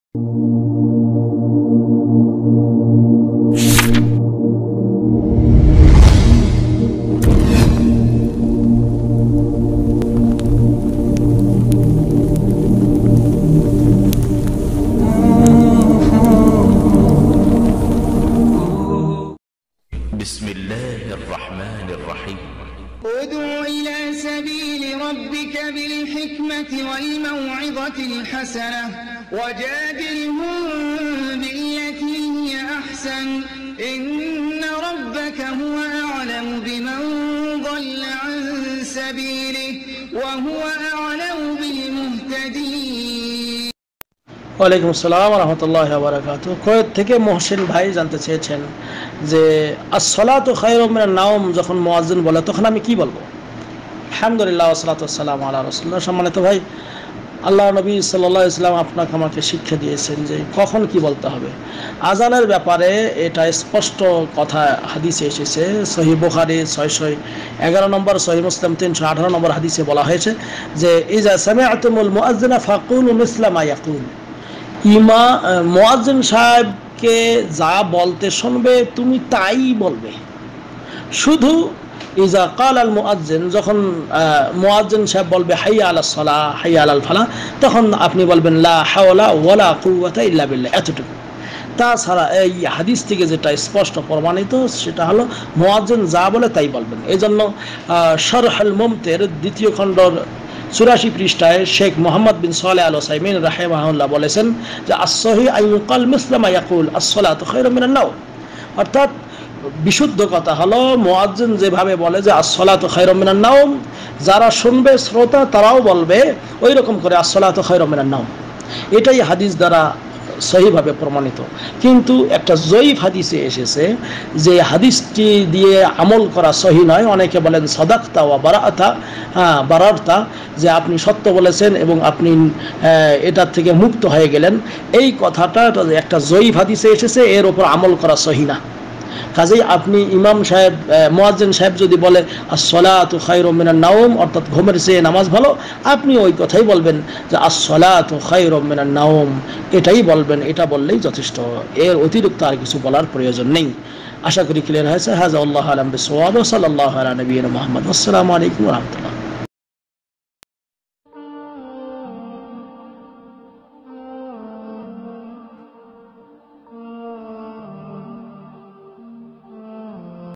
আজানে মুয়াজ্জিন 'আসস্বলাতু খায়রুন মিনান নাঊম' বললে আমাদেরকে কি বলতে হবে ?
🎤 উত্তর দিয়েছেনঃ- শাইখ আব্দুল হামিদ বিন সিদ্দীক হুসাইন সালাফি।

🎤 Answer/ Sheikh Abdul Hamid Siddique Hossain Salafi.